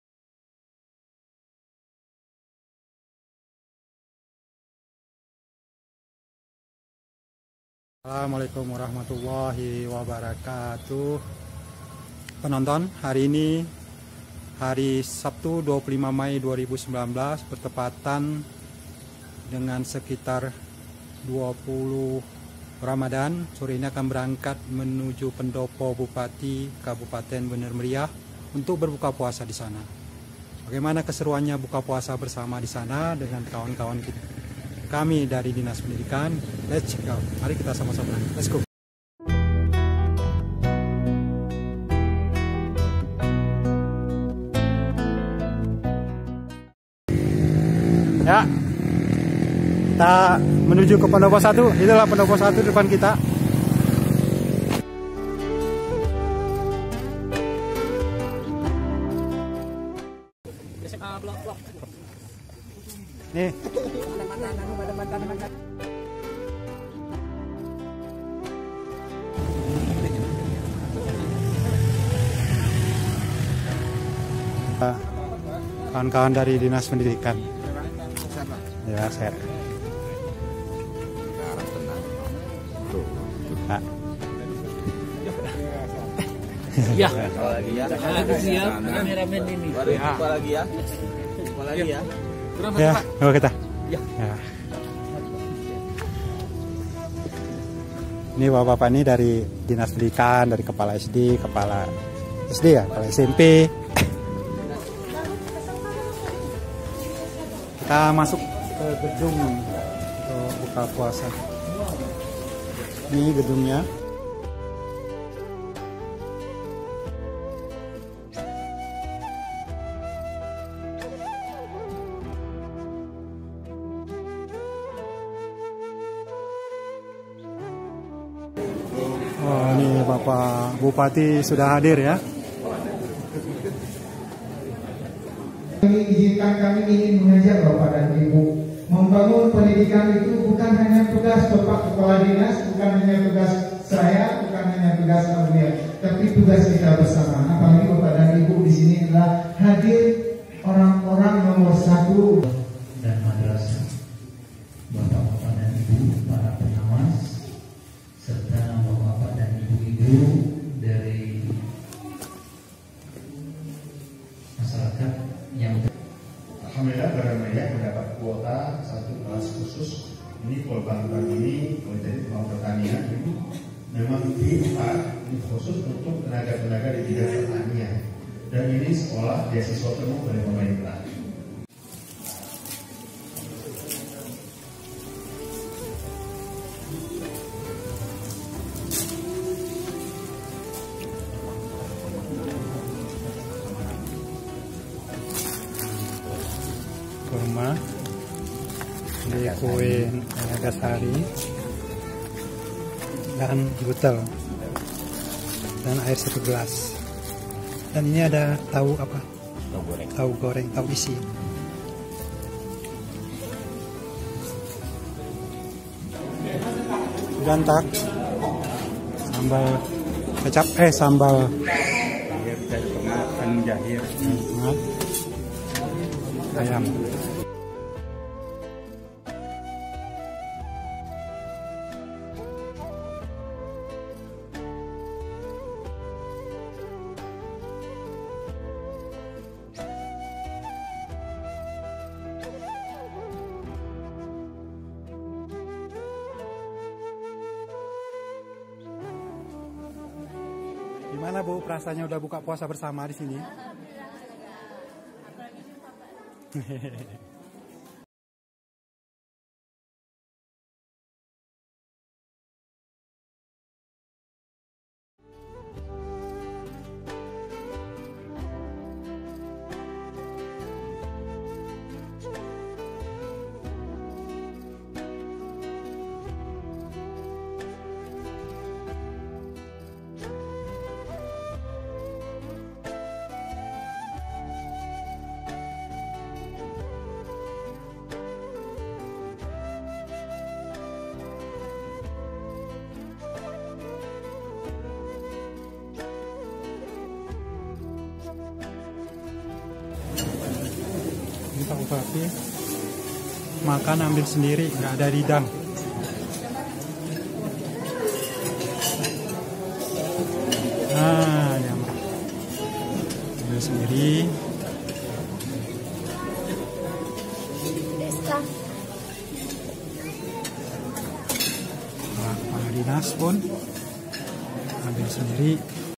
BUKBER PUASA KEPALA SEKOLAH DGN BUPATI BENER MERIAH
Bupati BM, Tgk. Syarkawi mengundang Dinas Pendidikan dan Kepsek SMP, SD SMA dan lainnya di pendopo 1.